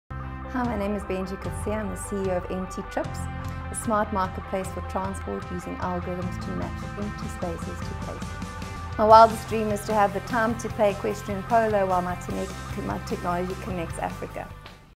Seedstars World South Africa 2017 | Empty Trips
Interview with Benji Coetzee, Founder and CEO of EmptyTrips